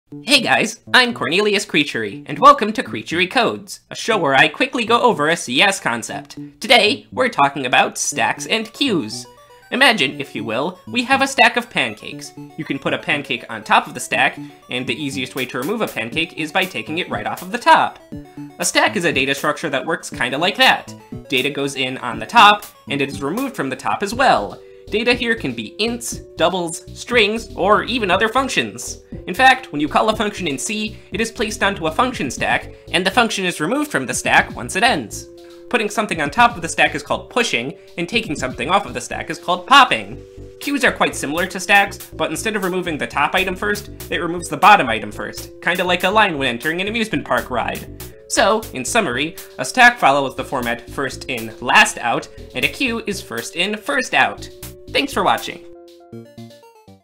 Stacks and Queues | Creaturey Codez
I finally got around to making that video about Stacks and Queues! I also wanted to see what that whole " " thing was all about.